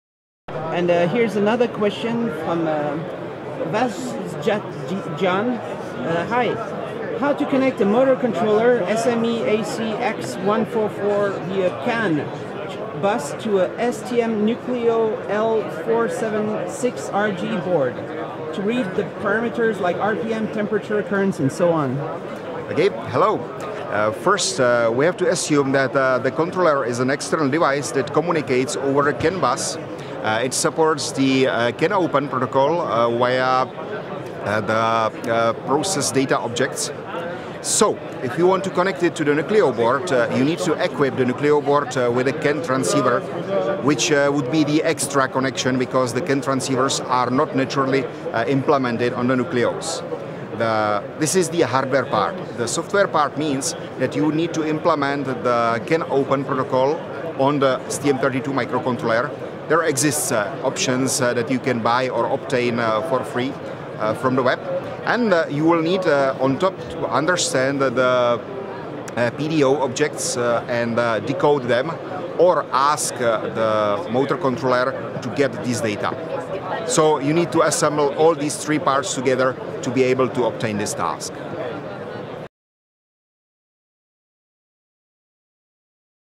askST EW2019: How can I connect a motor controller via a CAN bus to a NUCLEO-L476RG board?
vasjartigian
askST Hi! How can I connect a motor controller (sme ac-x144) via a CAN bus to an STM32L4 Nucleo board (NUCLEO-L476RG) to read the parameters such as rpm, temperature, currents and so on?